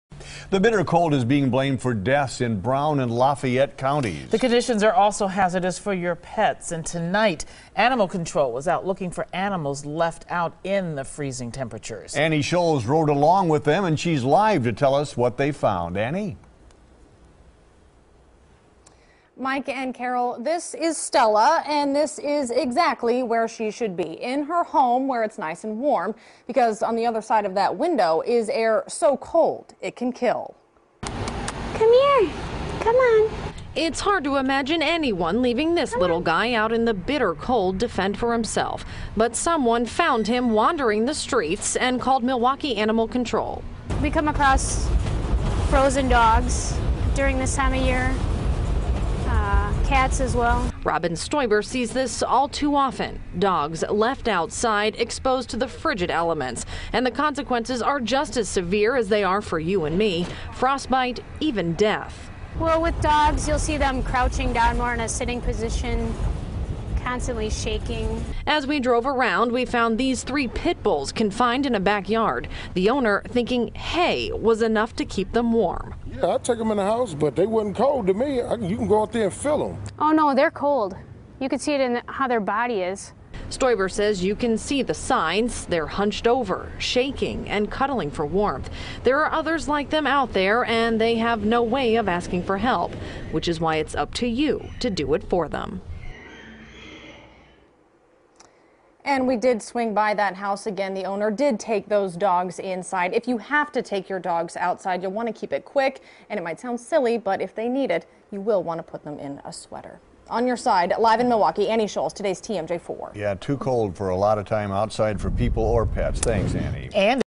Cold weather conditions can be hazardous for your pets
Annie Scholz reports